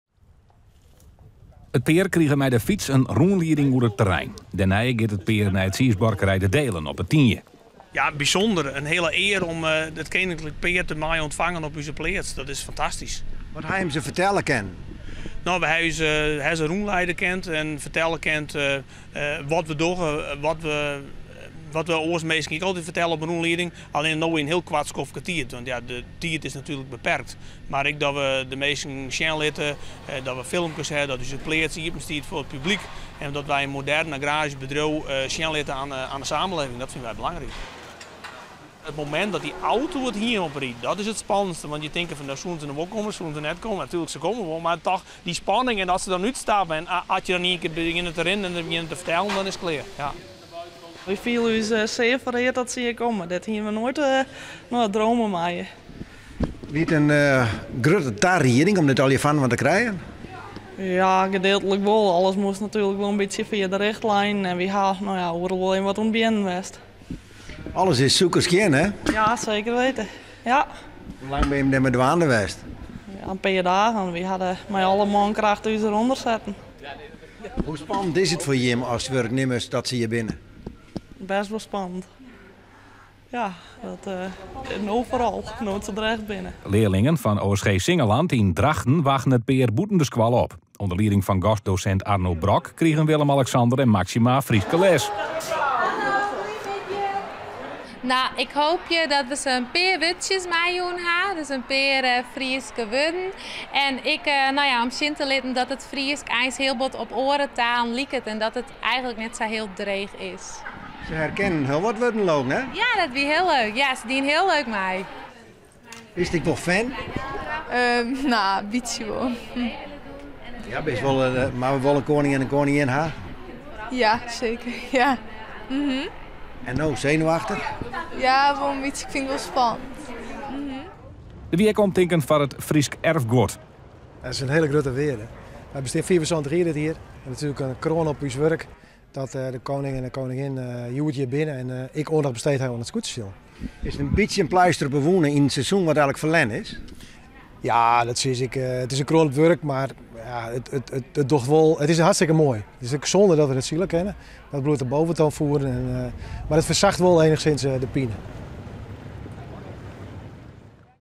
REPO: Koninklijk bezoek aan Fryslân
Koning Willem-Alexander en koningin Máxima waren donderdag de hele dag in het zuidoosten van Fryslân. Het is de eerste keer sinds de coronacrisis dat het koningspaar een streekbezoek brengt. Er werd onder andere gesproken over duurzaamheid en over de Regiodeal. Zuidoost-Fryslân is één van de twaalf regio's die extra geld krijgen om de regio te versterken. Het bezoek was anders dan anders vanwege het coronavirus. Het schema was daarom voor het grootste deel geheim, zodat er geen mensen op konden afkomen.